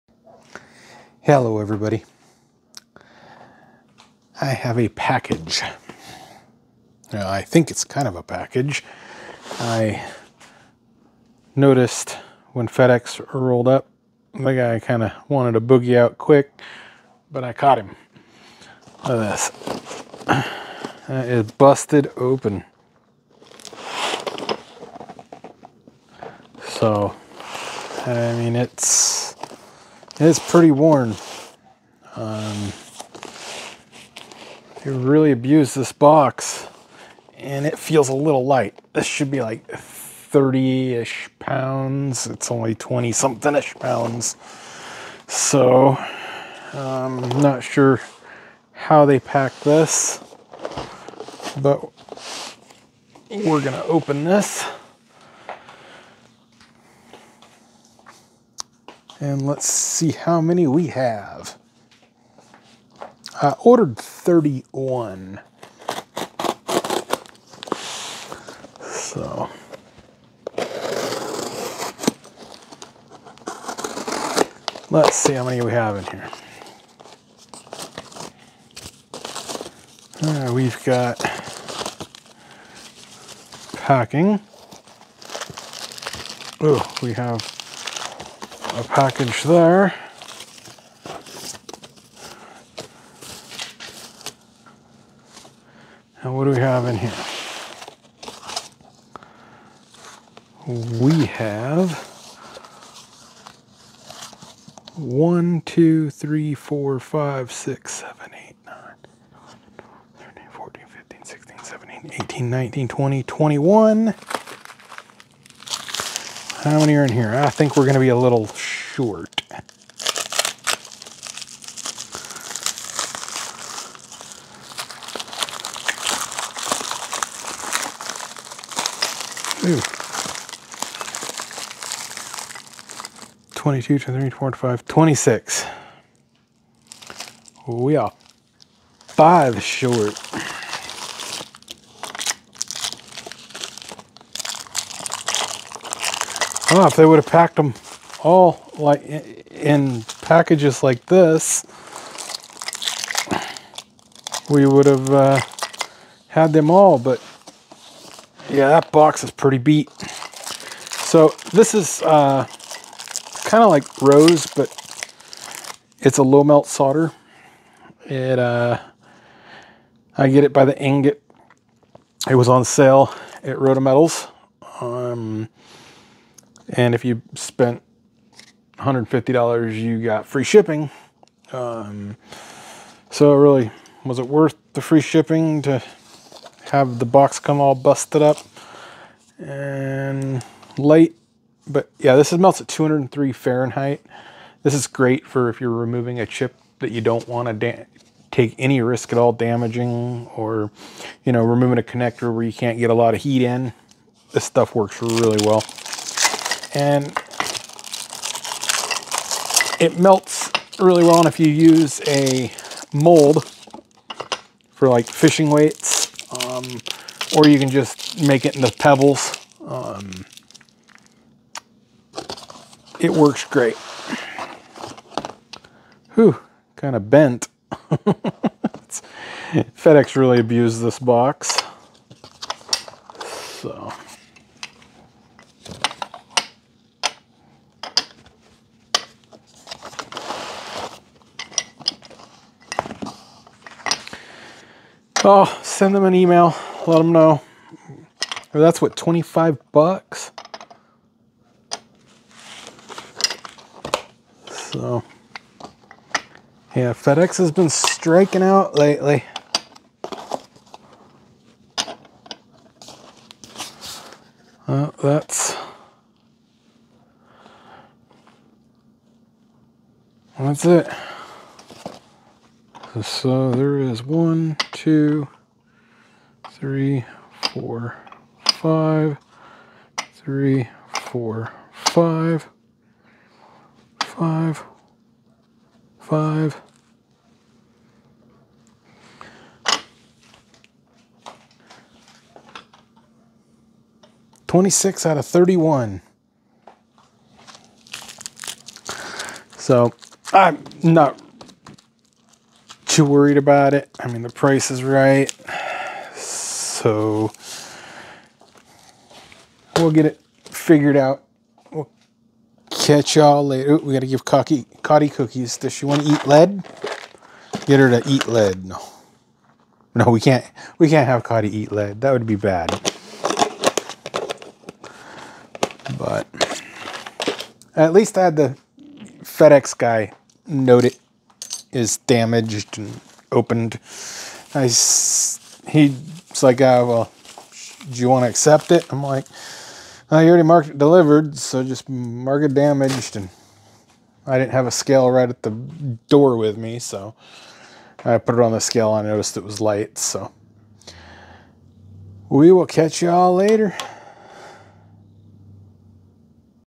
Lifetime Supply Low Melt Solder Roto Matals 203F 95C Ingots
Probably Multiple Lifetimes,

An ingot of Roto203F will average approximately 1 pound and consists of 52.5% Bismuth, 32% Lead, 15.5% Tin.

I Have been using this blend for a while, I Now buy in ingot Form form the Manufacture and it works well Cut your self off a small piece or melt it down poor in to fishing weight stick molds.

Rotometals sent the 5 missing pieces.